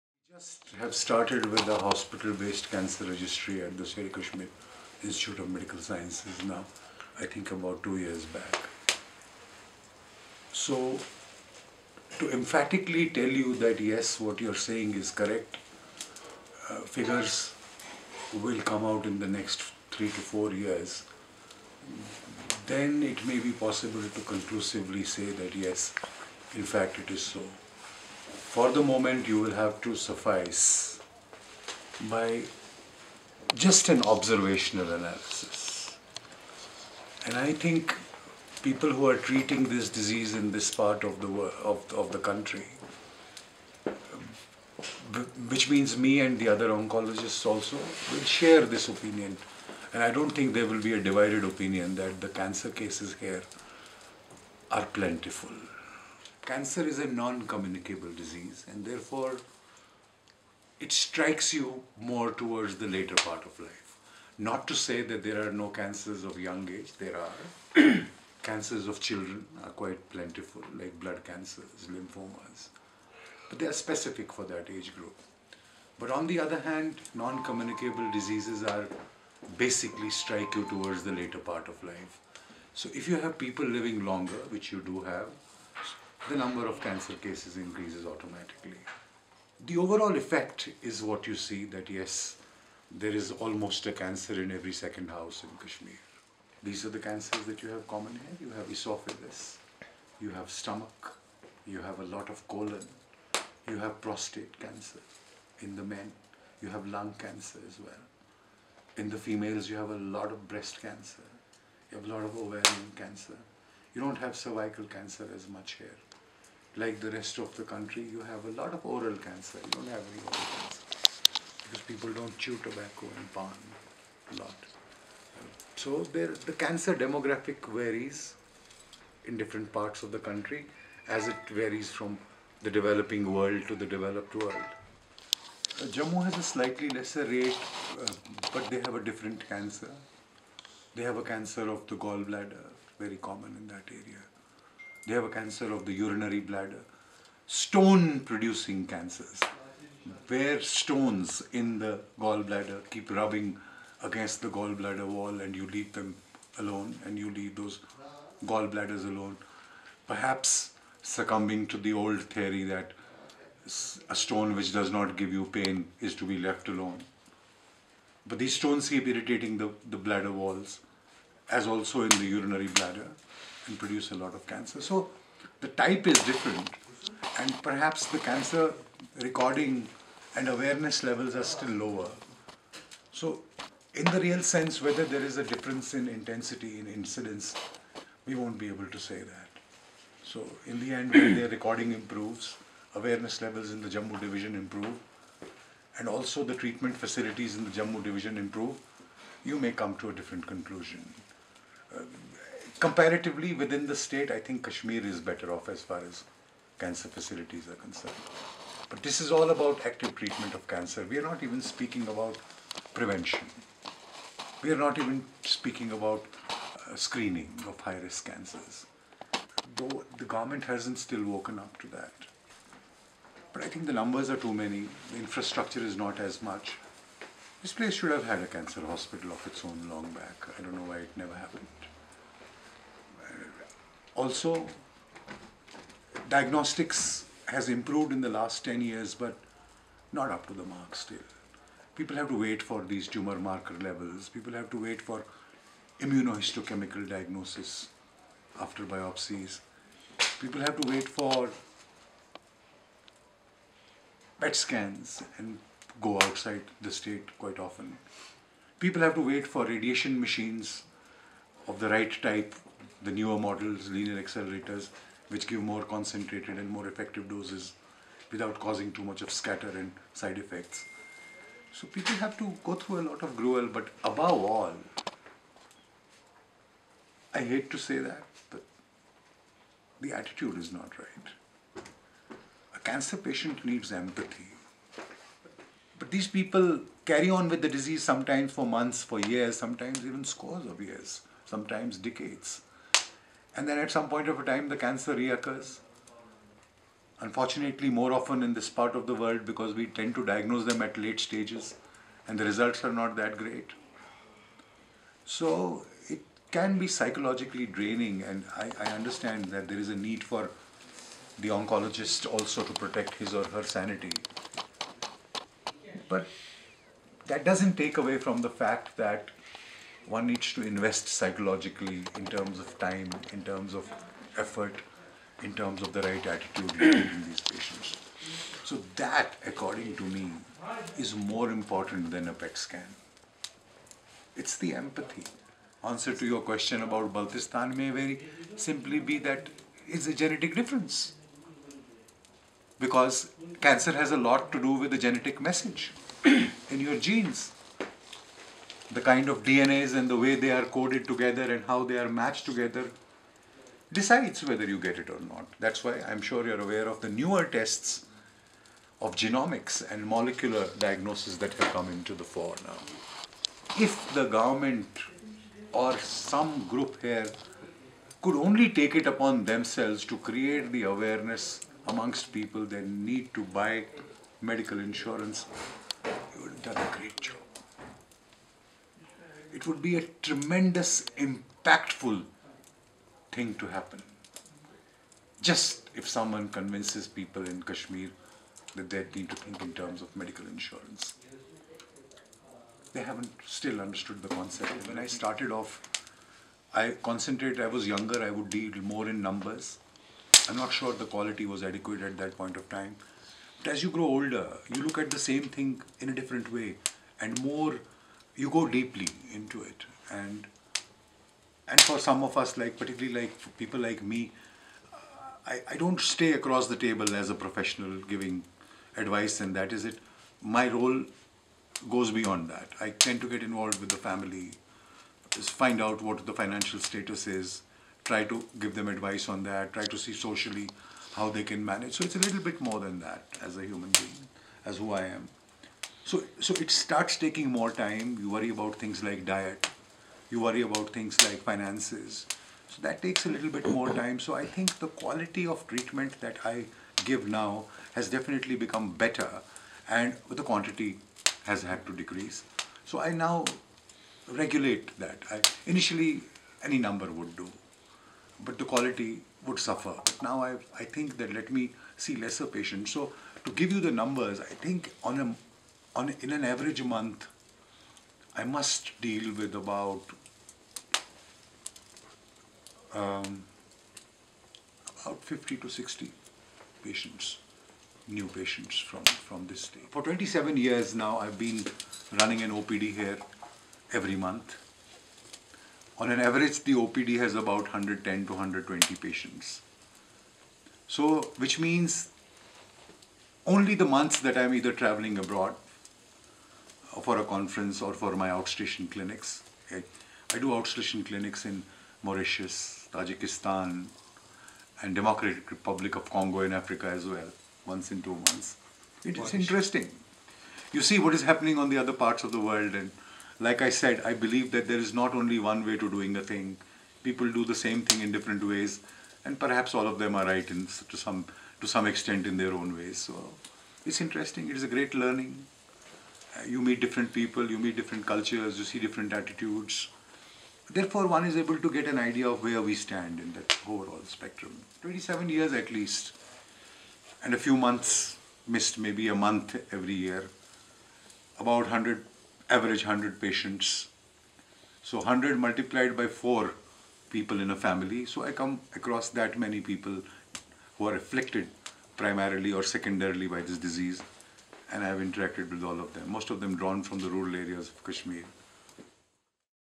Dr SAMEER KOUL ON CANCER
Kashmir cancer surgeon Dr Sameer Kaul talks about the incidence of the killer disease, its management and the deficit in infrastructure in the valley. He talks about the genetics and medical insurance.